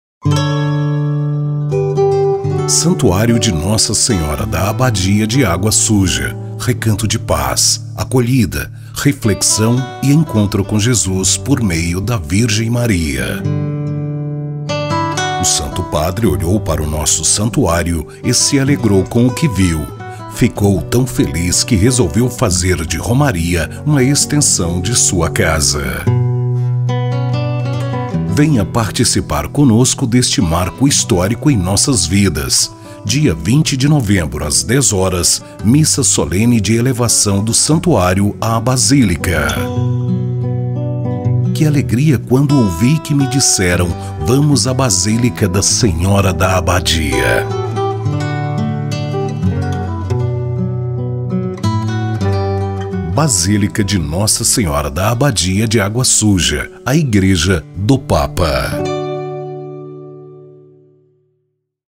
Santuário da Abadia de Romaria será elevado à Basílica Menor
No dia 20 de novembro de 2023, às 10h, acontecerá a Missa Solene de Elevação à Basílica, do então Santuário de Nossa Senhora da Abadia da Água Suja, em Romaria.